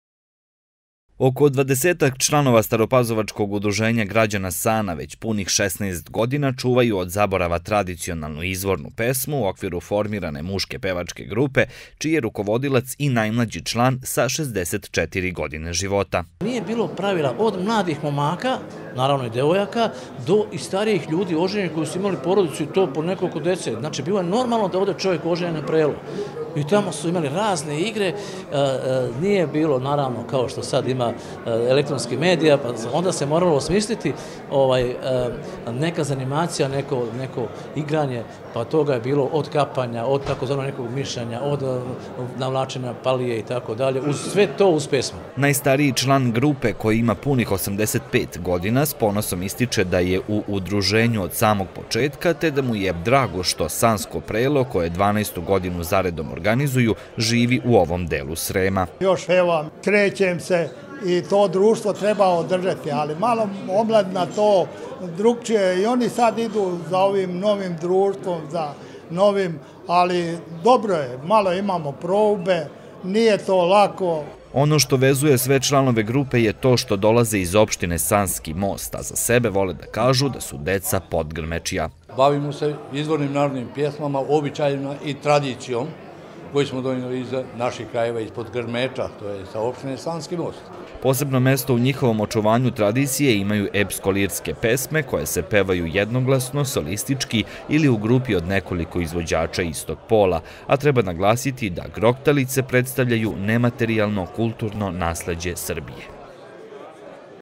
12. Sansko prelo u Vojki
Za mlade devojke i momke prela su nekada predstavljala mesto na kojima su se okupljali u jesenjim i zimskim večerima i najčešće su bila prpraćena šalom i smehom, a mladi moci delili su komplimente budućim udavačama. Danas se prela organizuju ne bi li se od zaborava očuvala tradicija. 12. Sansko prelo u organizaciji staropazovačkog UG „Sana“ održano je u Vojki, a pored domaćina predstavili su se članovi KUD-a „Dr Mladen Stojanović“ iz Bačkog Jarka, Krajiška grupa „Perućičani“ iz Vojke i članovi KUD-a „Mladost“ iz Nove Pazove. Događaju čiji je pokrovitelj opština Stara Pazova prisustvovali su predsednik opštine Đorđe Radinović i predsednik Saveza Srba iz regiona Miodrag Linta.